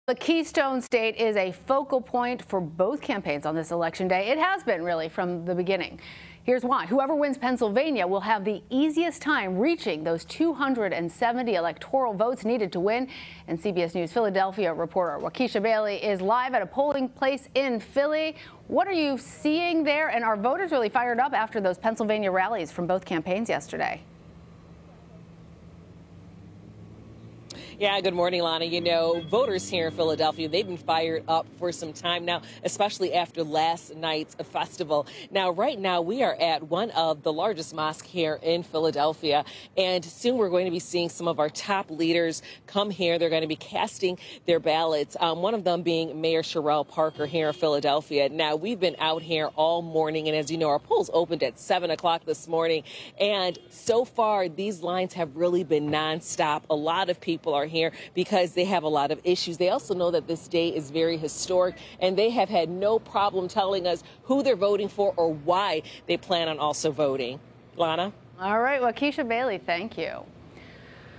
Philadelphia voters ready to cast ballots in battleground state: "Lines have really been nonstop"
Pennsylvania is one of the key battleground states this Election Day, evidenced by the multiple stops Vice President Kamala Harris and former President Donald Trump made there throughout their campaigns. CBS News Philadelphia reporter Wakisha Bailey has more from a polling place in Philadelphia.